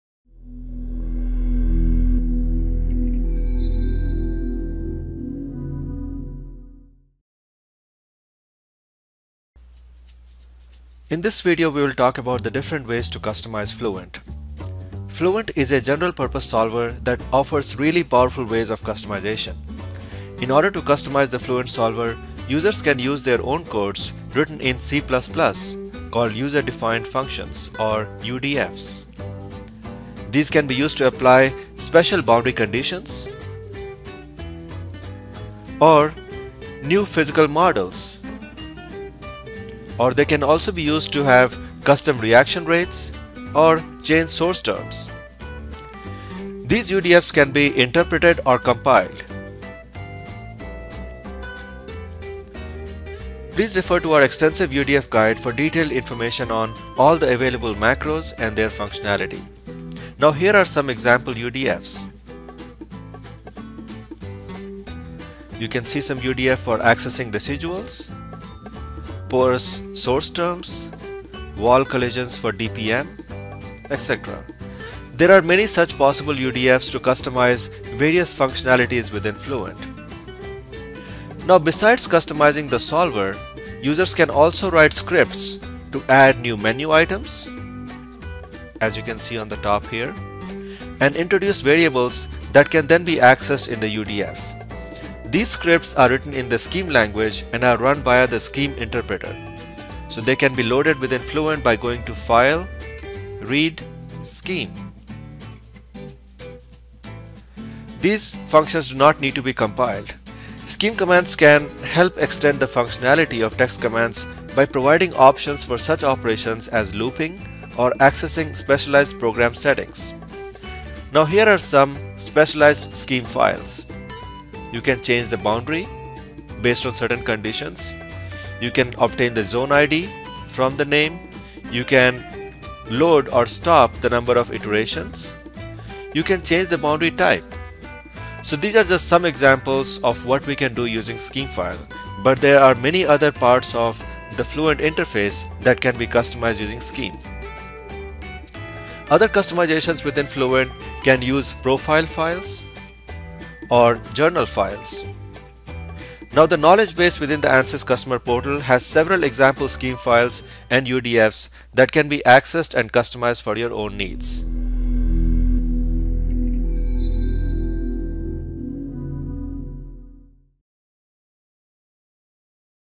What are the Different Ways to Customize Fluent
In this video, we will learn about the different ways in which we can customize Fluent. Fluent is a general purpose solver that offers really powerful ways of customization. These customization methods can be used to apply special boundary conditions, input solution dependent material properties, use new physical models, custom Reaction rates, change source terms etc.